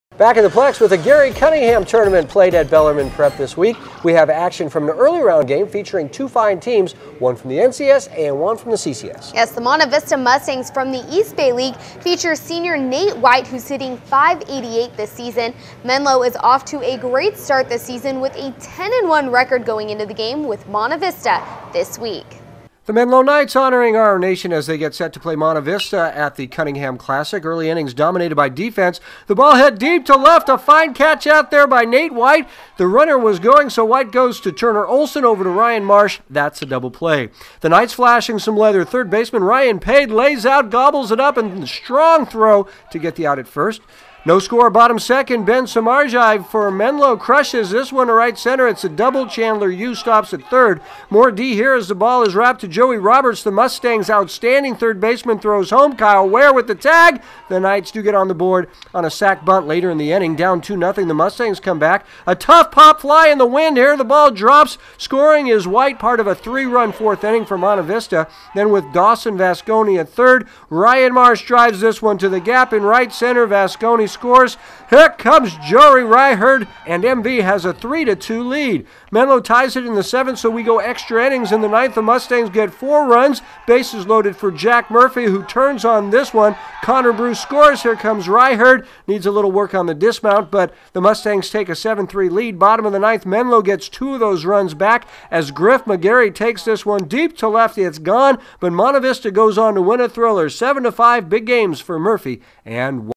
Monte Vista at Menlo Baseball
THE MENLO KNIGHTS HONORING OUR NATION AS THEY GET SET TO PLAY MONTE VISTA AT THE CUNNINGHMA CLASSIC.
EARLY INNINGS DOMINATED BY DEFENSE THE BALL HIT DEEP TO LEFT CENTER A FINE CATCH OUT THERE BY NATE WHITE THE RUNNING WAS GOING SO WHITE GOES TO TURNER OLSON OVER TO RYAN MARSH THAT’S A DOUBLE PLAY.
THE KNIGHTS FLASHING SOME LEATHER HERE AS THIRD BASEMAN RYAN PADE LAYS OUT GOBBLES UP THE BALL AND STRONG THROW TO GET THE OUT AT FIRST.
NO SOCRE BOTTOM SECOND BEN SOMORJAI FOR MENLO CRUSHES THIS ONE TO RIGHT CENTER IT’S A DOUBLE CHANDLER YU STOPS AT THIRD.
MORE D HERE AS THE BALL IS RAPPED TO JOEY ROBERTS THE MUSTANGS OUTSTANDING THIRD BASEMAN HE THROWS HOME KYLE WEHER WITH THE TAG THE KNIGHTS DO GET ON THE BOAD ON A SAC BUNT LATER IN THE INNING.
DOWN TWO NOTHING THE MUSTANGS COME BACK A TOUGH POP FLY INTO THE WIND HERE THE BALL DROPS SCORING IS WHITE PART OF A THREE RUN FOURTH INNING FOR MONTE VISTA.
THEN WITH DASWON VASCONI AT THIRD RYAN MARSH DRIVES THIS ONE TO THE GAP IN RIGHT CENTER VASCONI SCORES HERE COMES JOEY RIHERD AND MV HAS A THREE TO TWO LEAD.
MENLO TIES IT IN THE SEVENTH SO WE GO TO EXTRA INNINGS, IN THE NINTH THE MUSTANGS GET FOUR RUNS BASES LOADED FOR JACK MURPHY WHO TURNS ON THIS ONE AND LINES A SINGLE TO RIGHT CONNOR BRUCE SCORES HERE COMES RIHERD NEEDS A LITTLE WORK ON THE DISMOUNT, BUT THE MUSTANGFS TAKE A 7-3 LEAD.
BOTTOM OF THE NINTH MENLO GETS TWO OF THOSE RUNS BACK AS GRIFF MCGARRY TAKES THIS ONE DEEP TO LEFT IT IS GOEN BUT MONTE VISTA GOES ON TO WIN A THRILLER 7-5 BIG GAMES FOR MURPHY AND WHITE..